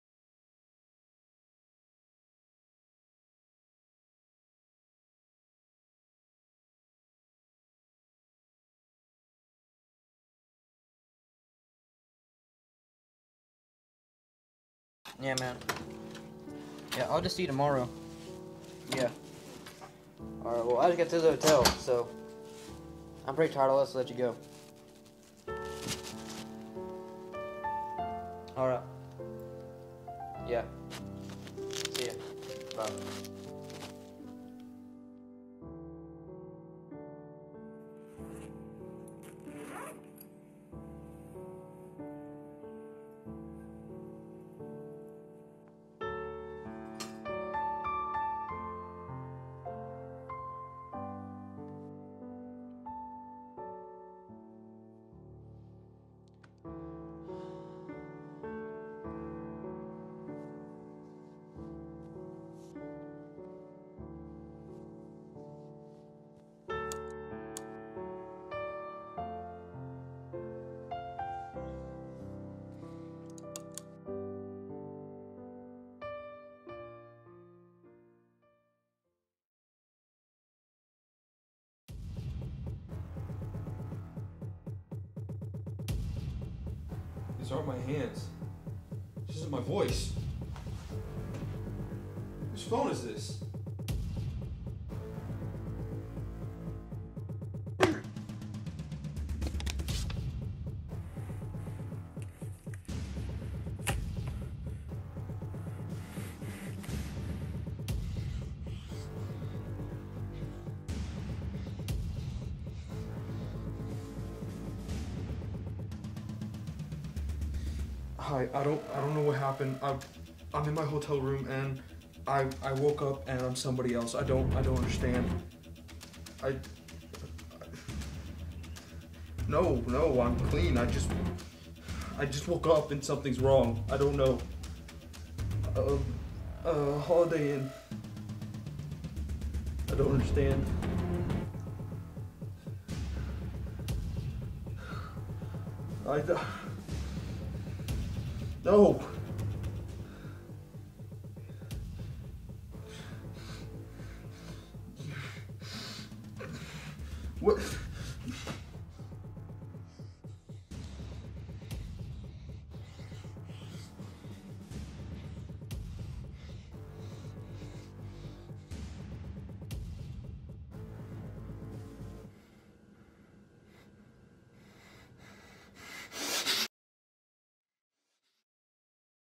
Parallel Universe (SkillsUSA Digital Cinema Production District 5 2020 - Team F)
This is Tyler Snell with Seth Spivey from Crandall high school, this is our digital cinema production video. The prompt was “ Parallel Universe” we did not place top 3 out of 17. I think we got disqualified